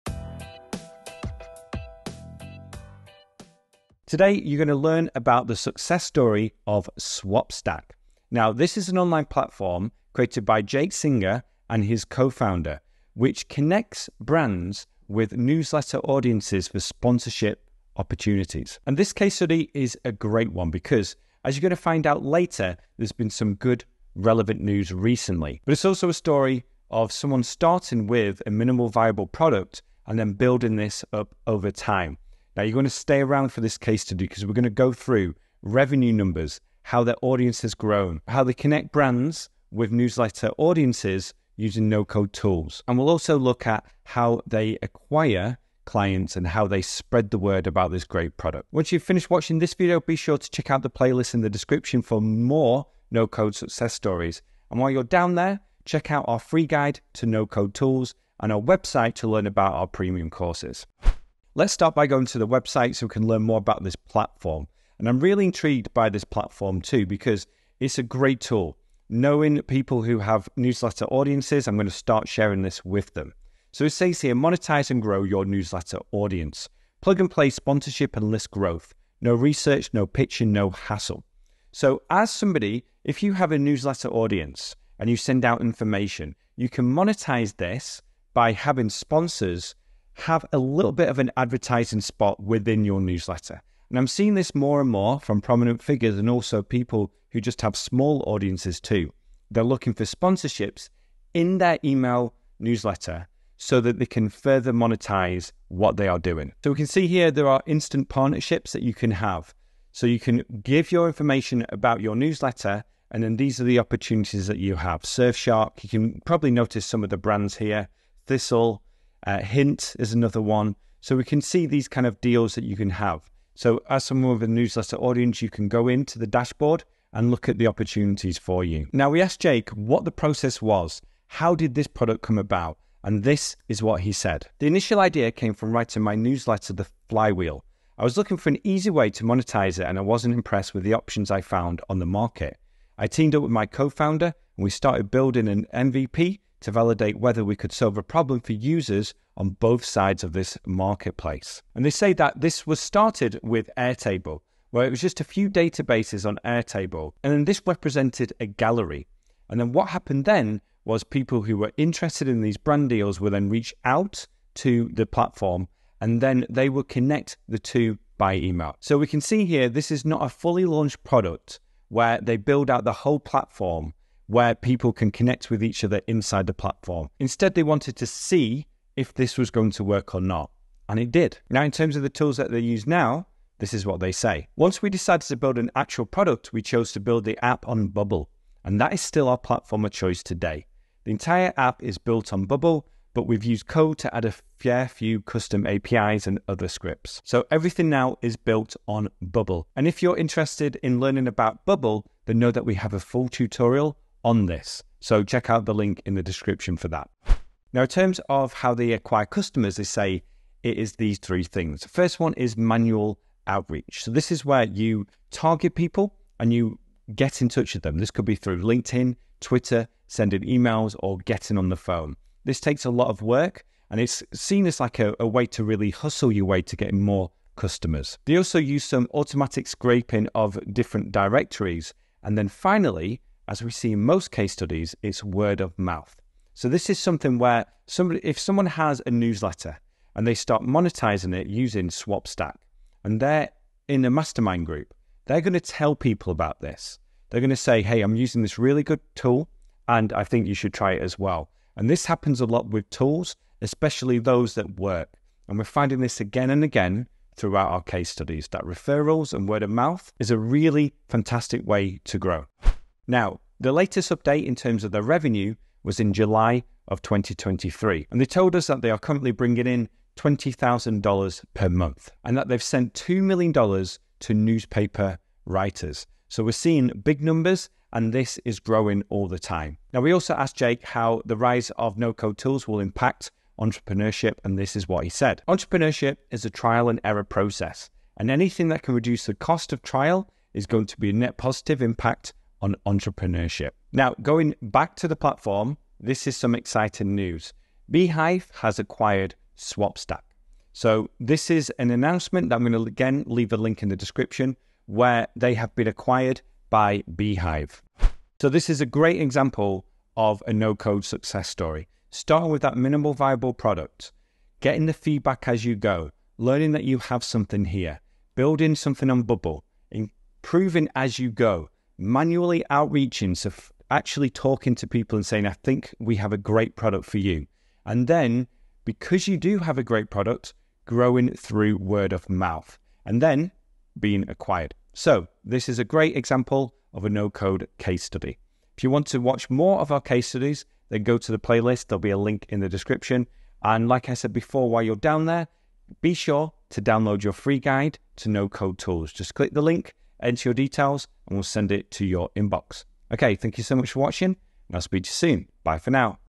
Incredible no code success story: from a mvp to being acquired (+ revenue numbers)
In this video, you're going to learn about the success story of SwapStack - a marketplace for brands and newsletter audiences.

We'll go through their product, the tools they use, how they monetize, their revenue numbers, their marketing strategy, and more.

⭐ TAKE OUR FULL COURSE ON BUILDING APPS USING AI

CHAPTERS:

00:00 introduction to Swapstack
01:00 the product
03:01 the tools
03:27 getting customers
04:22 revenue numbers